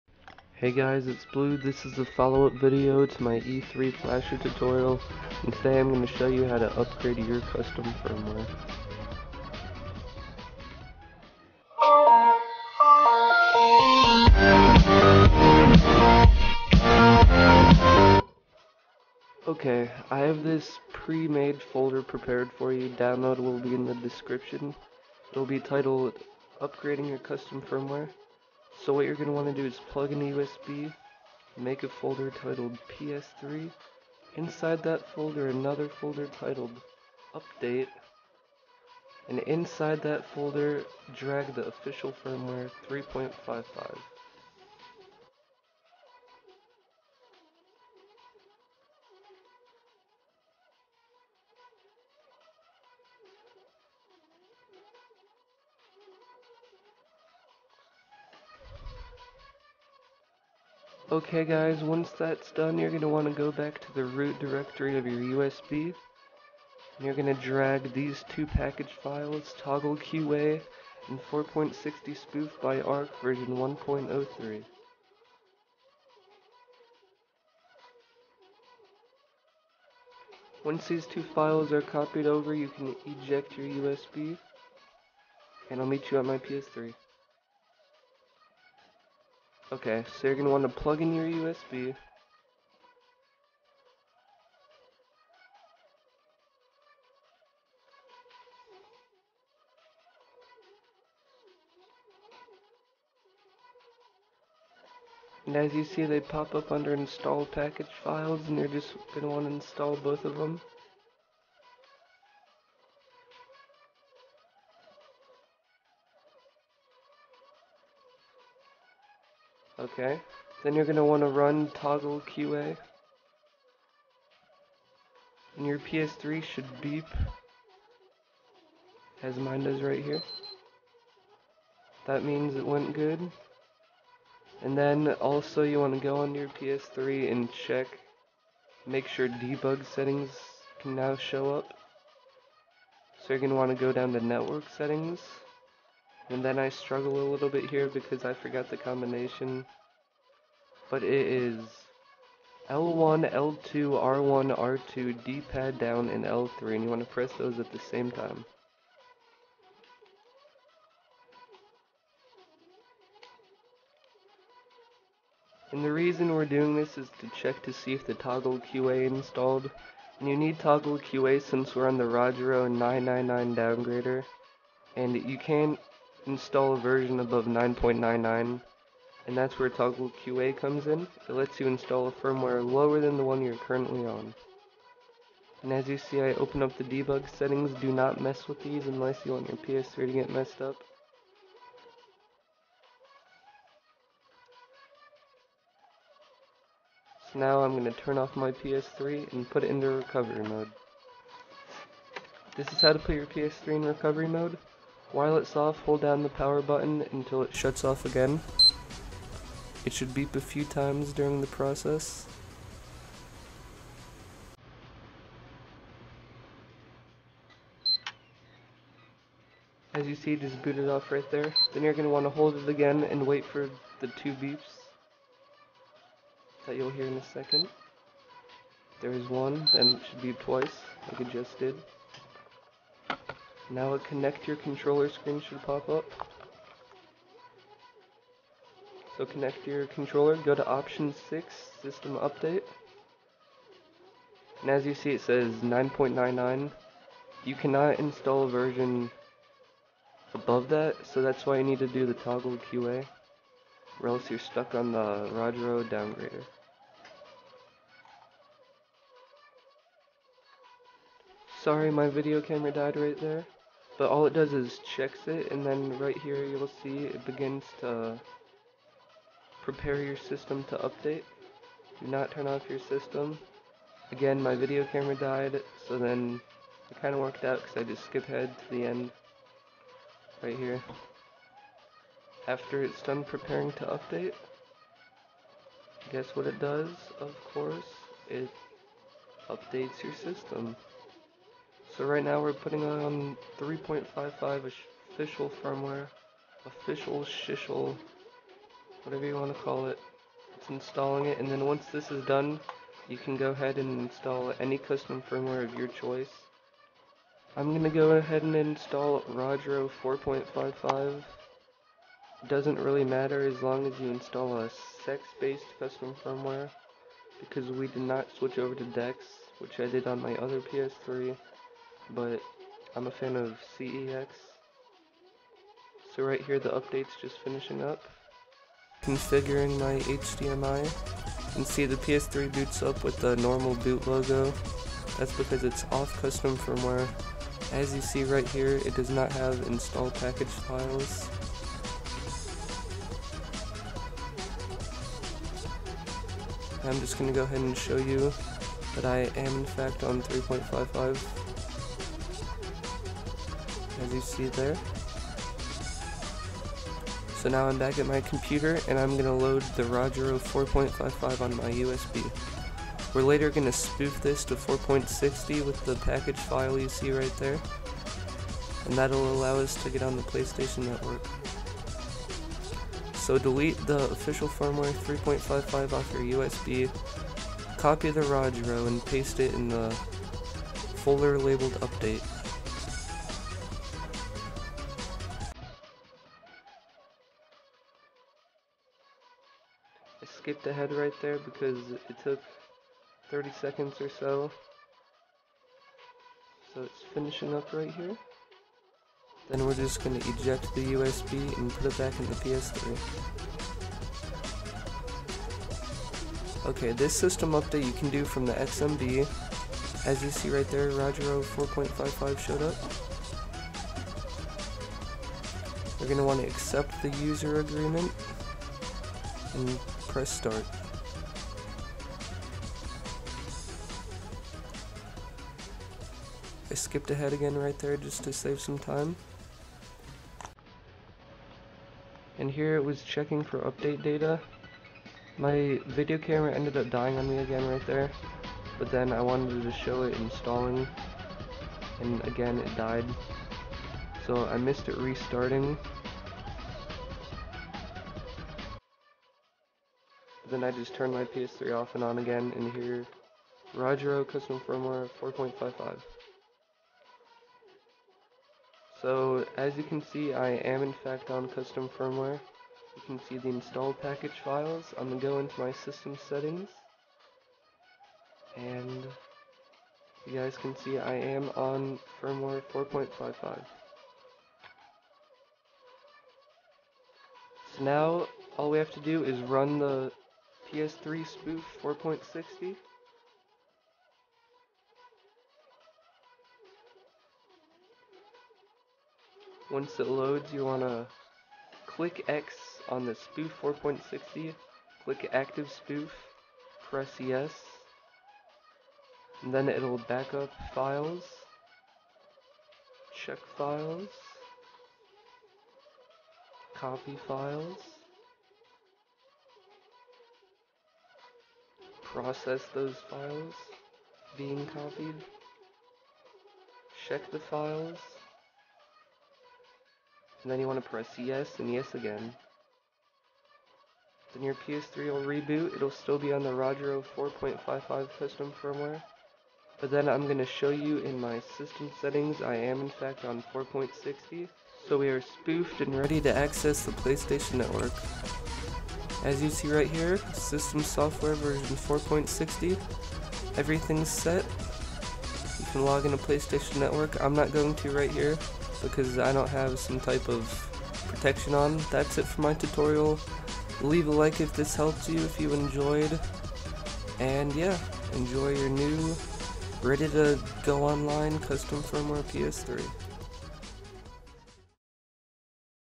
How to update PS3 CFW from Rogero downgrader !
Hey foxicans it's blu this is my followup video to my e3 flsher tutorial.

Please note: I am not responsible if you mess up your ps3. Not reading information WILL hurt you. Know what your doing.